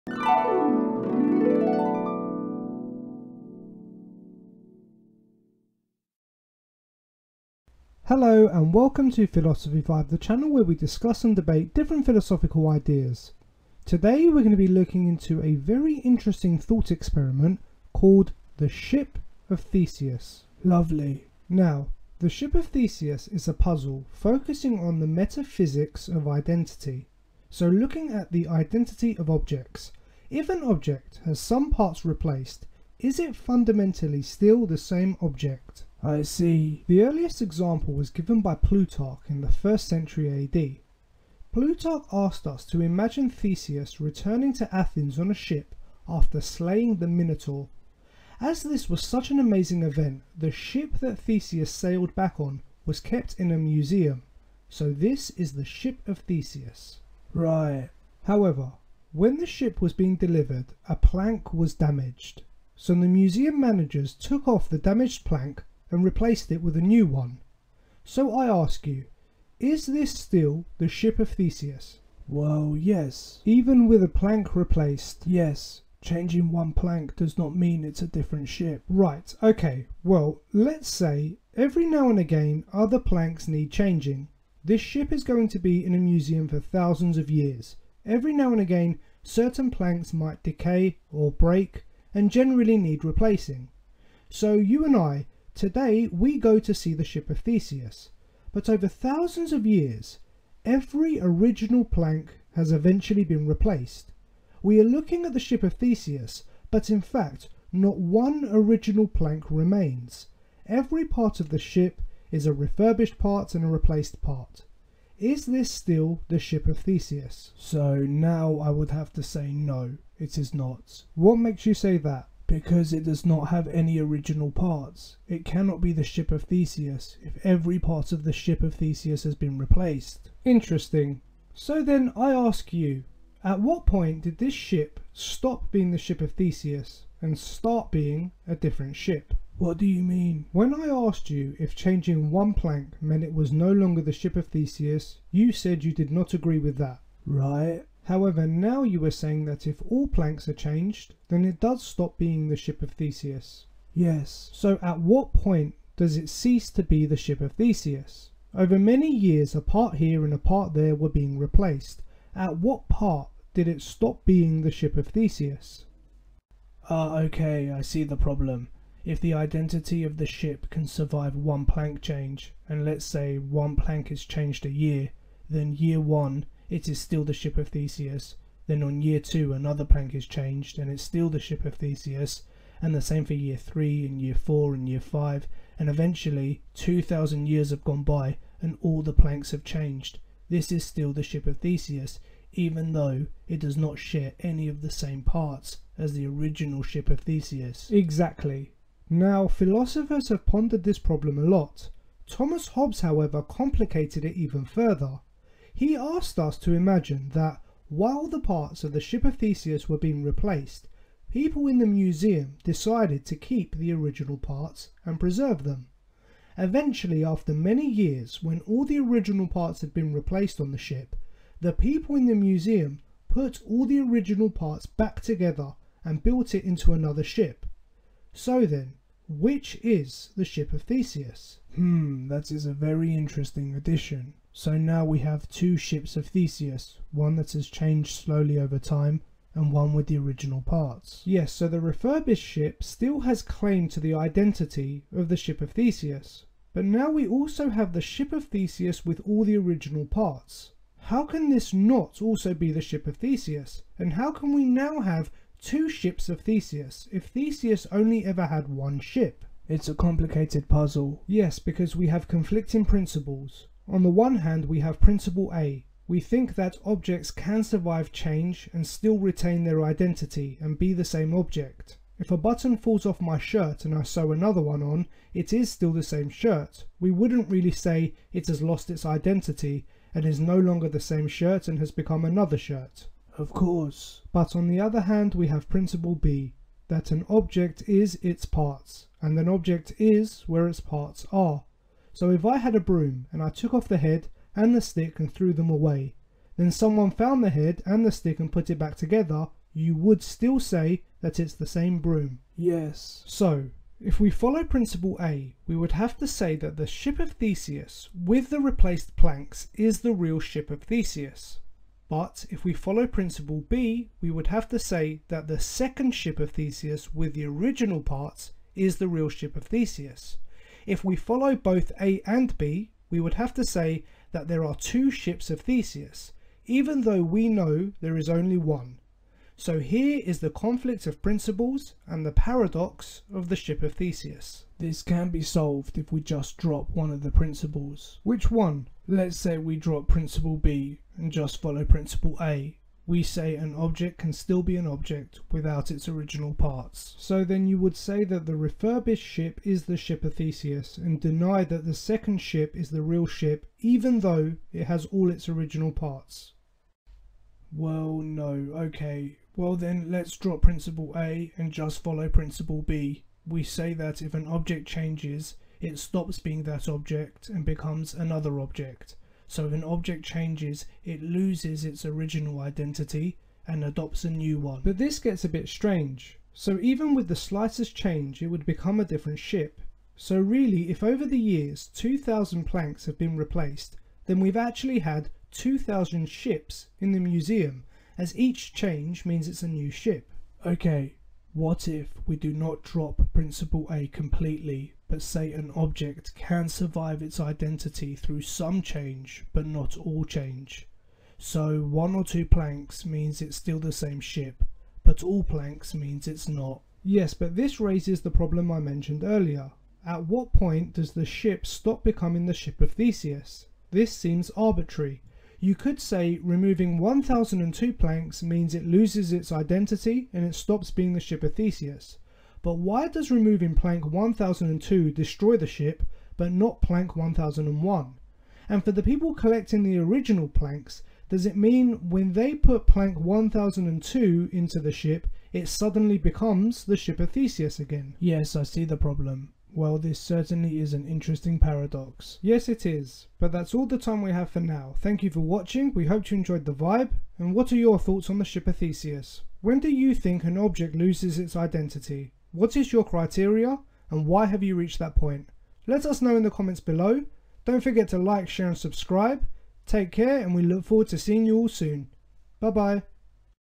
The Ship of Theseus (Metaphysics of Identity)
Join George and John as they discuss and debate different Philosophical ideas, today they will be looking into the Ship of Theseus.

This is a very interesting thought experiment pondering upon the metaphysics of identity. What gives an object its identity? Consider the Ship of Theseus, a famous ship that has been on display in a museum for thousands of years, over time the ship has needed maintenance, repair and refurbishment. However after two thousand years in the museum we find that today all the original parts have been replaced with newer parts.

So is this still the same ship or a different ship altogether?

Watch the debate and discover the paradox.